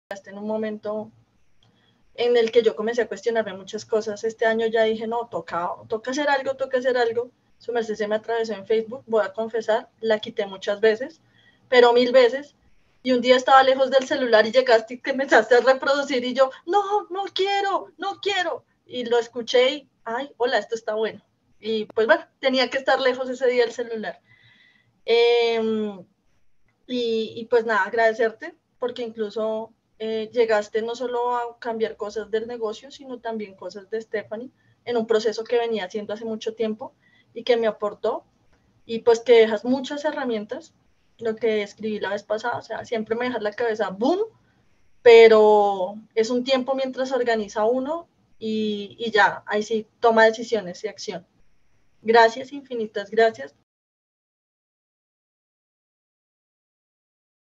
🤗 Conoce la historia de Stephany Ramírez
📌 Hoy te contamos la historia de Stephany Ramírez, quien perteneció al programa EmprendeSST y hoy tiene mejores resultados en su Negocio de Asesorías de Seguridad y salud en el trabajo, tú también lo puedes lograr.